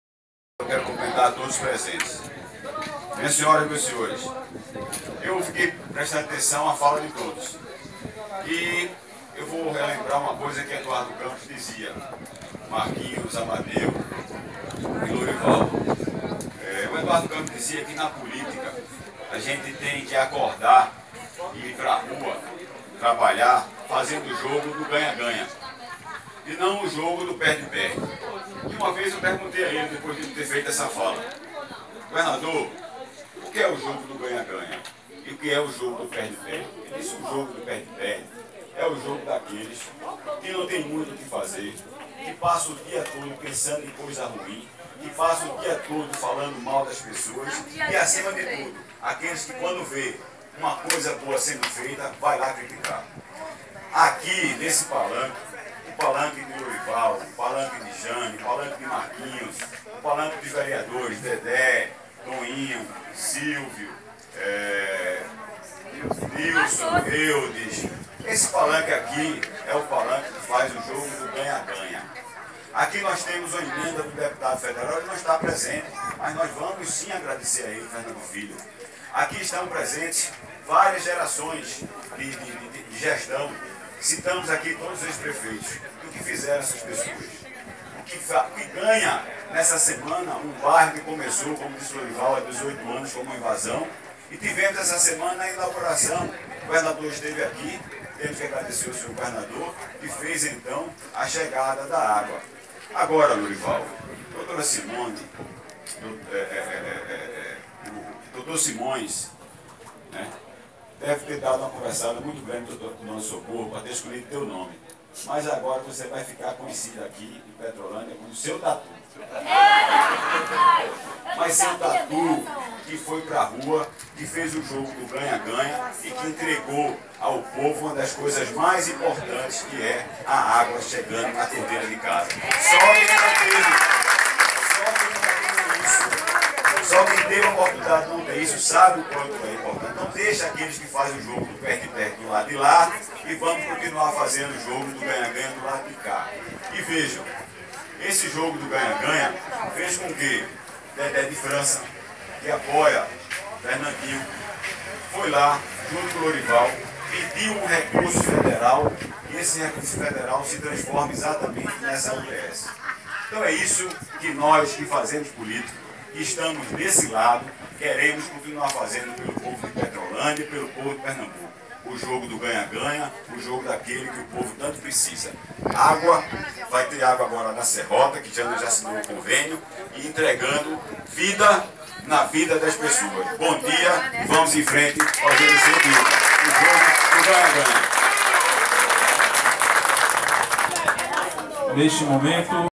Discurso do deputado estadual Alberto Feitosa durante inauguração da UBS do Bairro Nova Esperança
Blog de Assis Ramalho